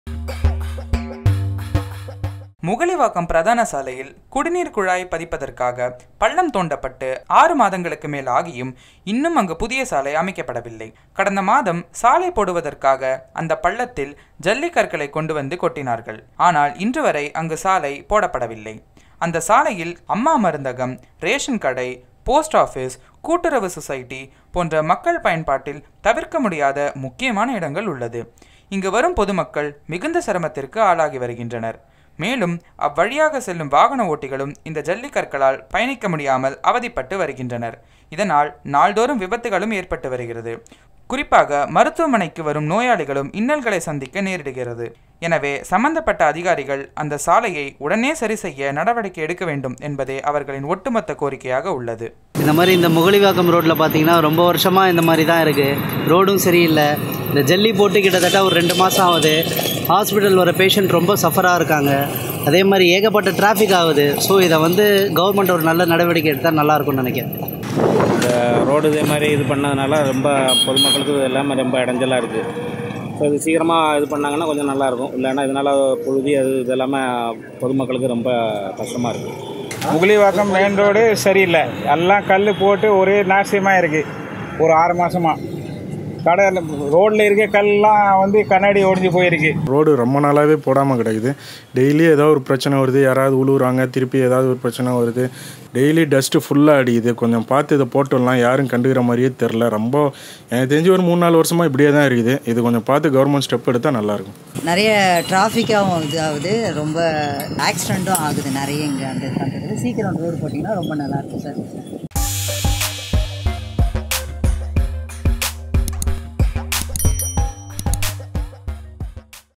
Roadu Fulla Jalli karkal | Porur Talk
The Metrowater dug up the stretch on Mugalivakkam Main Road to lay underground pipes. Six months have passed and the authorities laid only gravels on it. As a result, motorists and public are affected and many accidents are being reported. They request the authorities to
re-lay the road soon.